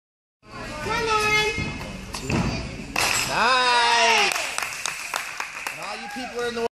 Lauren Gist - Triple flight series (beam)
Lauren Gist Beam
Backhandspring-BackHandspring-Back layout